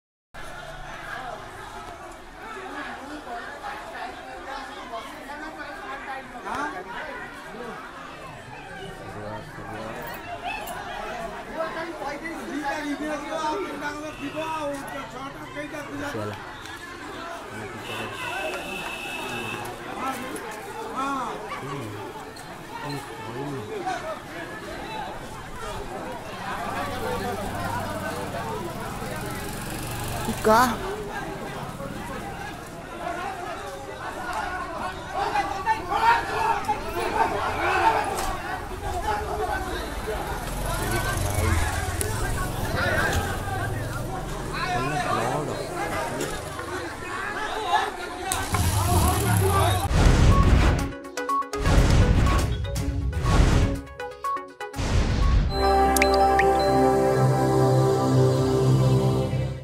Video of Polling booth Rigging in Dasharathpur, Jajpur.
Video of  Polling booth rigging in Ward 5 of Chital Panchayat, Dasharathpur, Jajpur.